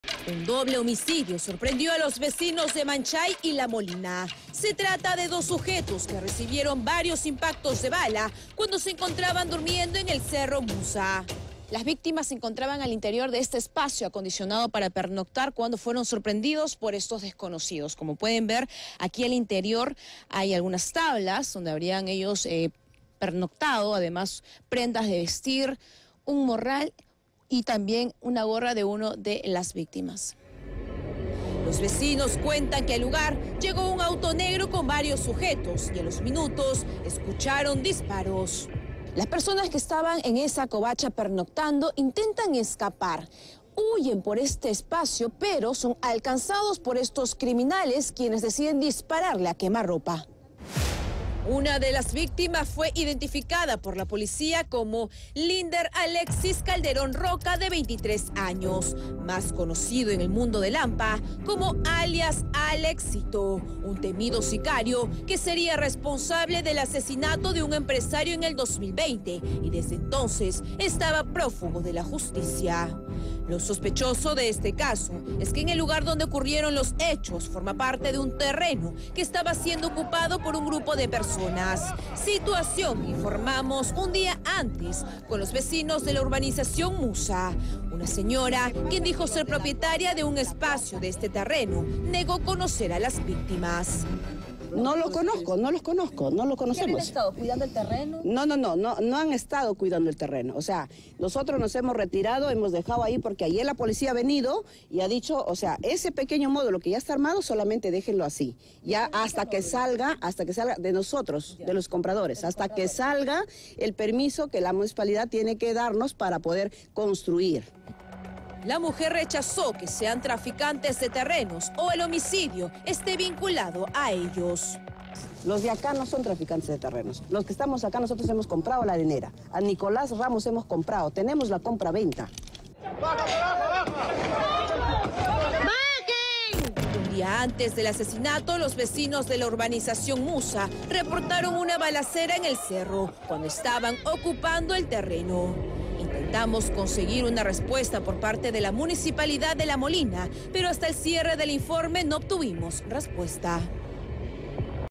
La Molina: uno de los sujetos asesinados en cerro tenía antecedentes policiales
En horas de la mañana dos hombres fueron asesinados en un cerro que limita entre La Molina y Manchay a manos de un grupo de personas que llegaron en un auto negro.

Según las primeras investigaciones que se han realizados, el crimen estaría relacionado al trafico de terrenos entre las zonas de Ate y La Molina.

ANTECEDENTES

De acuerdo a la información brindada por los agentes de la PNP, una de las víctimas que encontró su muerte responde al nombre de Linder Calderón Roca (23) quien contaba con antecedentes policiales.

Finalmente, en las próximas horas se espera un pronunciamiento de la Municipalidad de La Molina para que puedan determinar la hipótesis del asesinato.